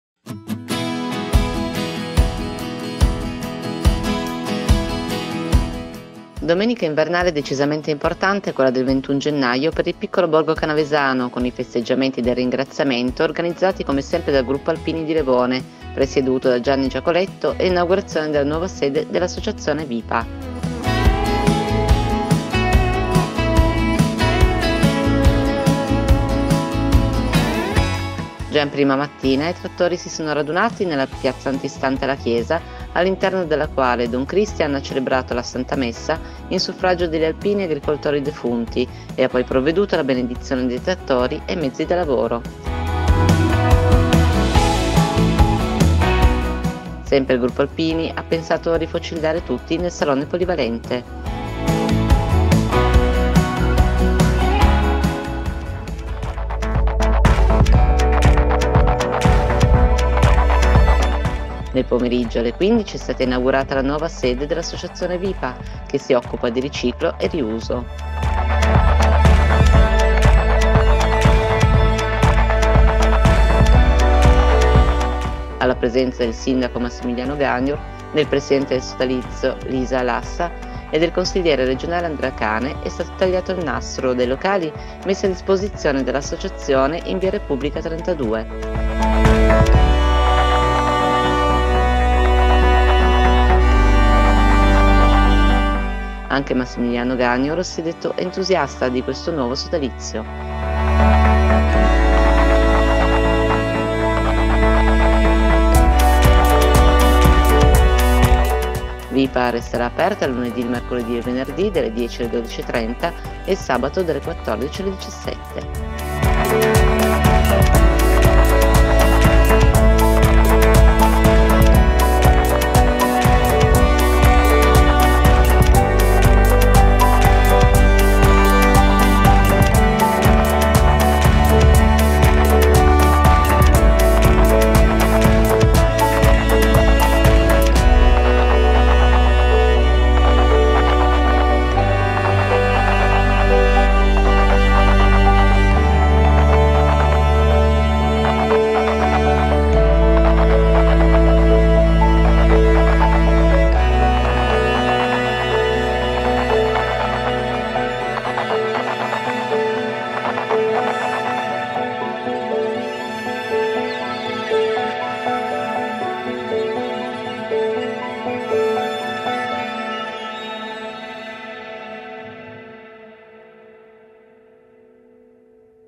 LEVONE –Domenica importante: ringraziamento ed inaugurazione della nuova sede dell'Associazione VIPA
Oggi, 21 gennaio 2024, è stata una domenica invernale decisamente importante quella per il piccolo borgo canavesano con i festeggiamenti del ringraziamento, organizzati come sempre dal Gruppo Alpini di Levone presieduto da Gianni Giacoletto, e l'inaugurazione della nuova sede dell'Associazione VIPA.

Il link dell'articolo sul nostro giornale